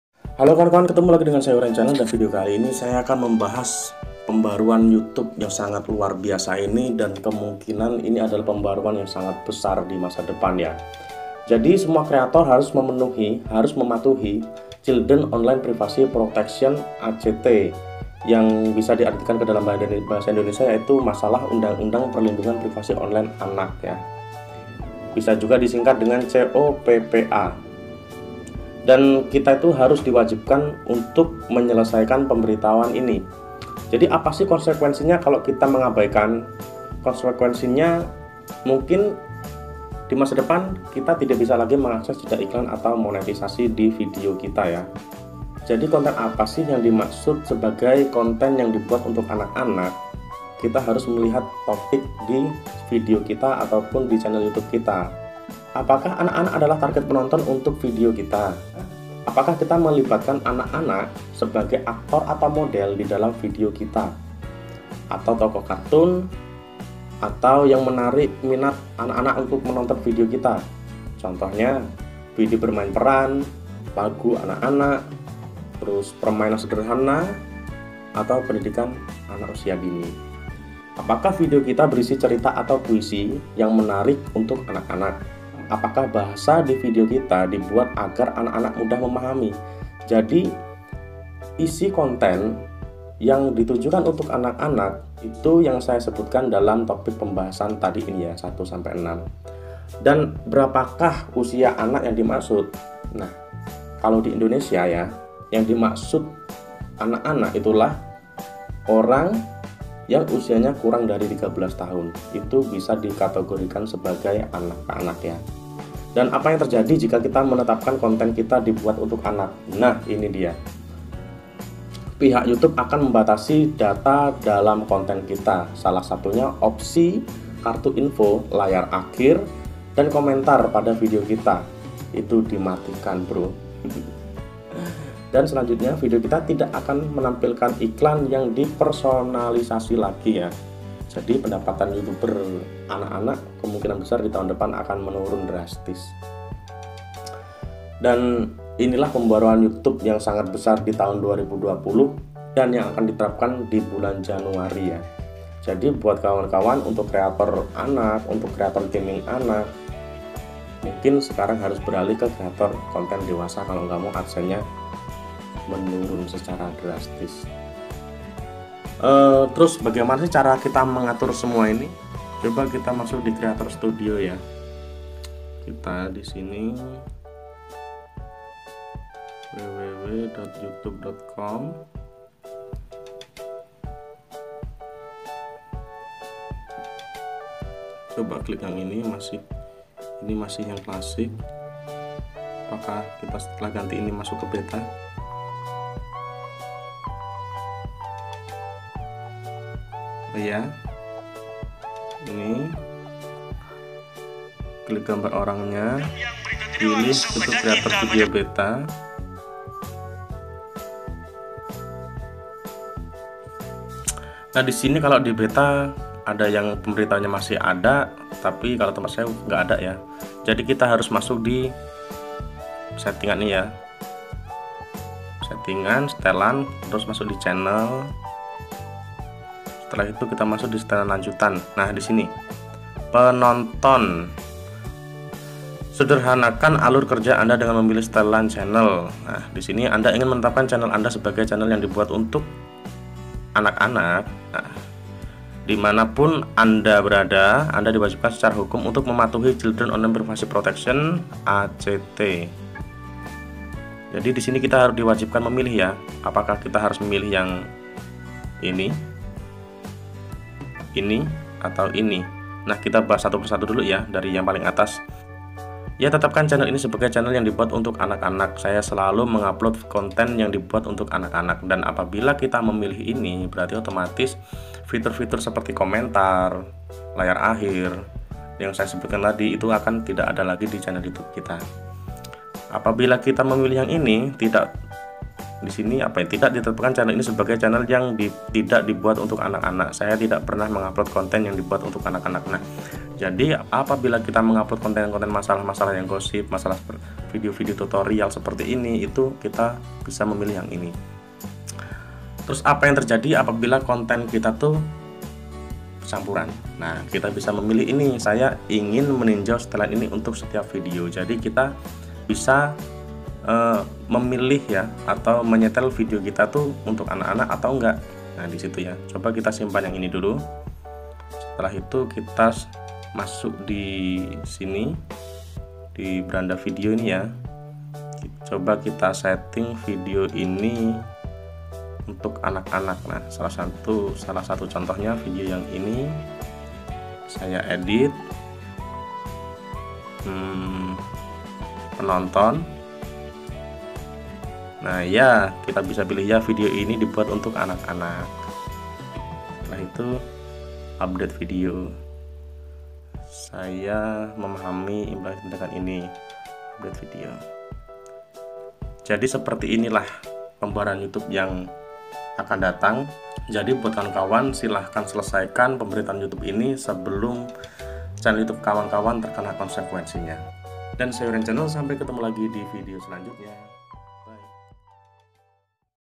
Pembaruan YouTube Tentang COPPA
Pembaharuan YouTube terbaru memang saat ini belum di terapkan, namun di tahun depan kita harus di wajibkan menetapkan channel kita sebagai channel yang dibuat untuk anak-anak atau bukan.